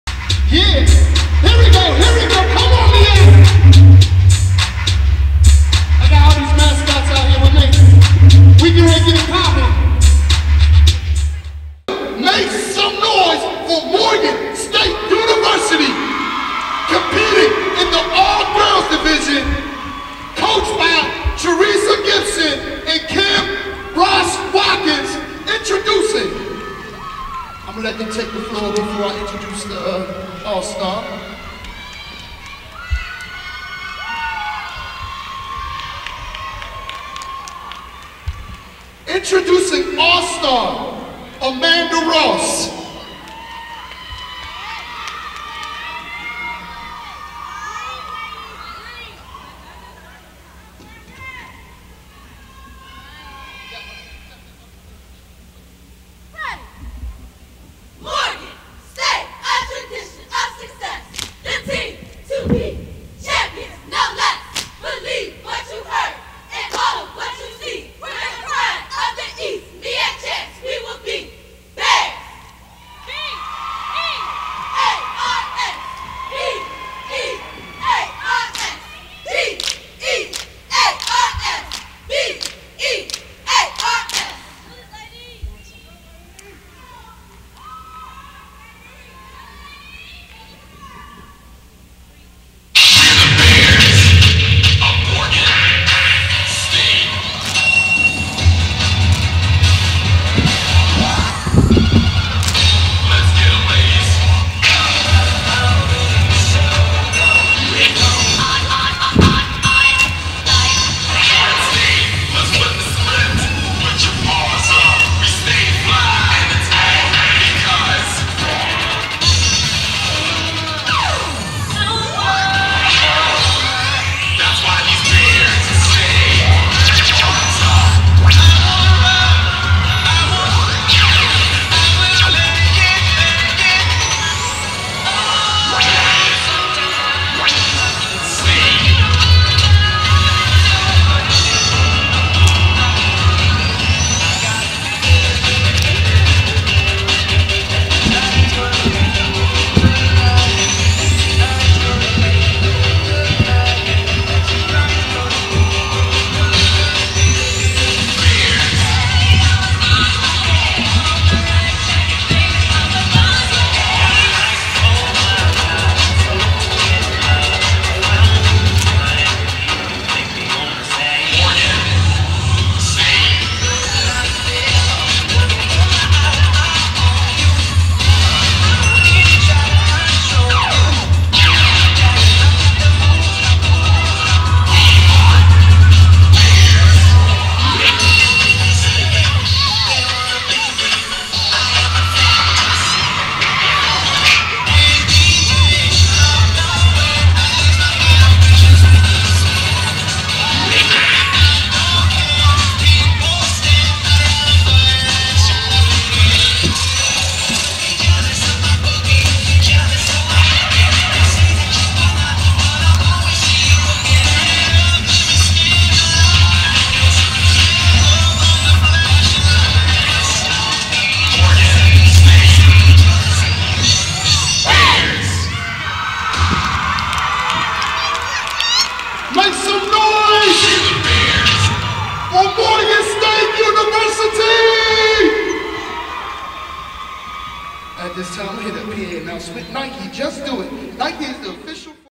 Morgan State at 2012 MEAC Cheerleading Competition
The Lady Bears compete defending their title